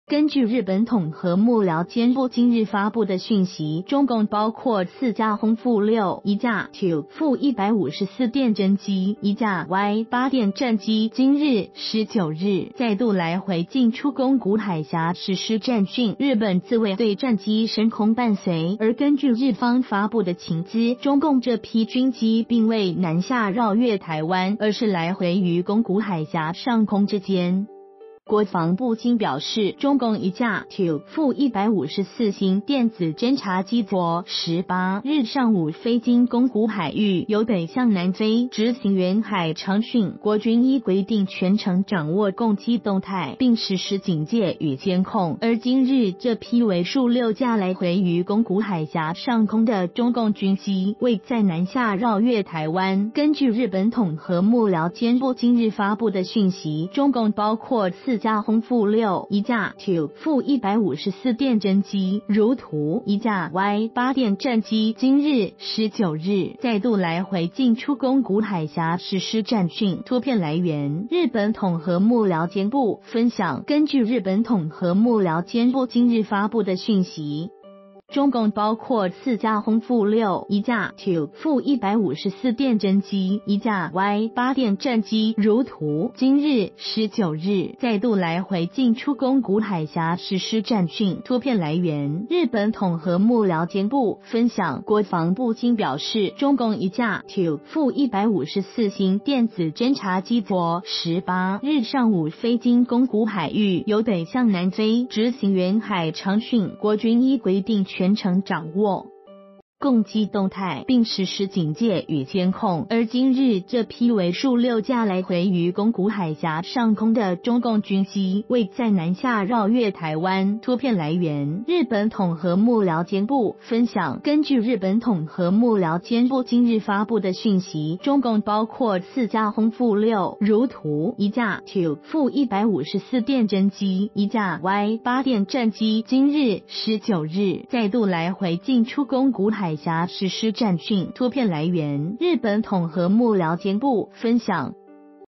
中共6軍機今進出來回宮古海峽 未南下繞越台灣
根據日本統合幕僚監部今日發布的訊息，中共包括4架轟-6、1架Tu-154電偵機、1架Y-8電戰機，今日（19日）再度來回進出宮古海峽實施戰訓，日本自衛隊戰機升空伴隨。而根據日方發布的情資，中共這批軍機並未南下繞越台灣，而是來回於宮古海峽上空之間。

發表評論評論如下。